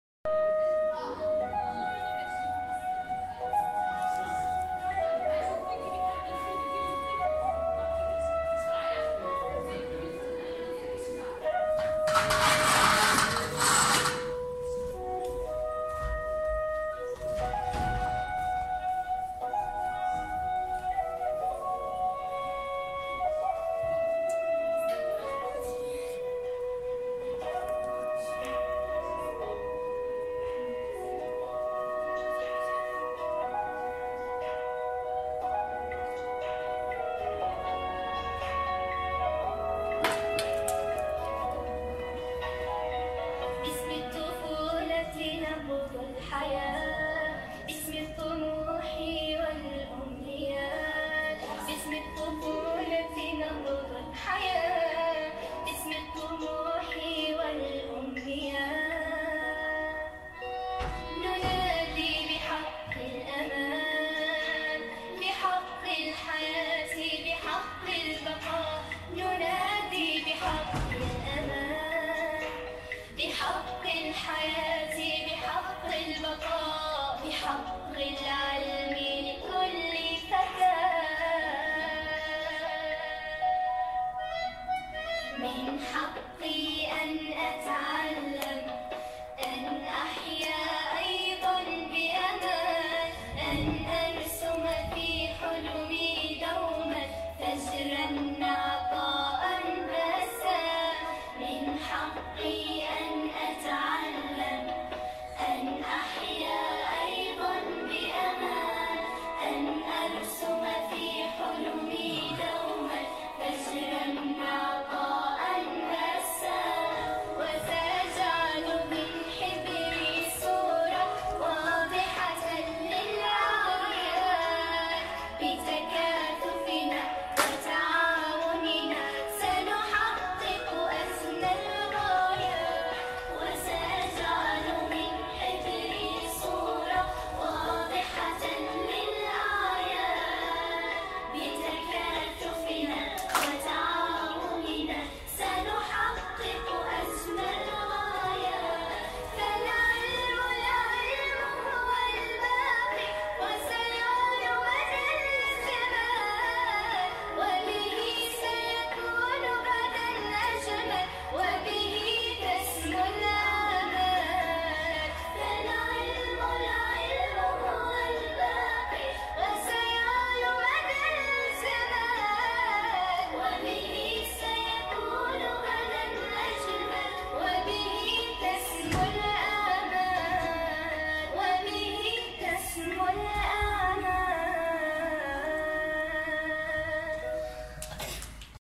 براعم مبادرة ارسم ابتسامه من أجمل المشاركات في فعاليات مبادرة لمبه الخامسه روعه
مشاركة براعم مبادرة ارسم ابتسامه خلال فعاليات مبادرة لمبه الخامسه فندق الفنار